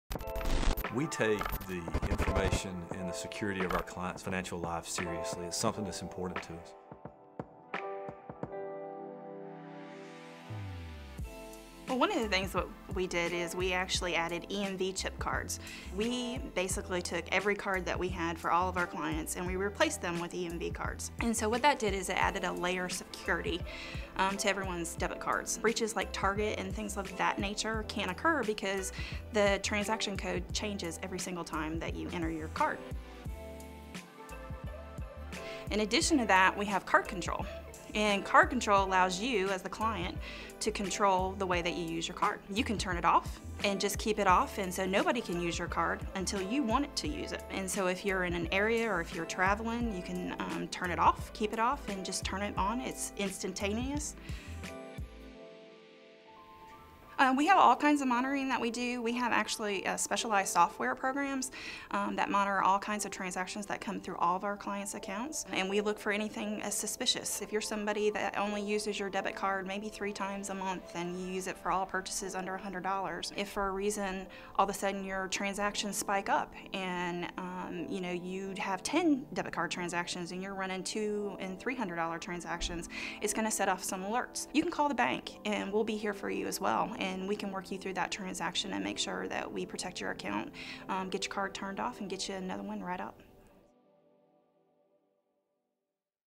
Secure & Protect - Dana White
Peace of mind should extend to all aspects of your banking relationship. That's why the expert team at AB&T is committed to offering a variety of products and services like EMV chip cards and Card Control to better safeguard what matters most. Security. Protection. Confidence. We Got It.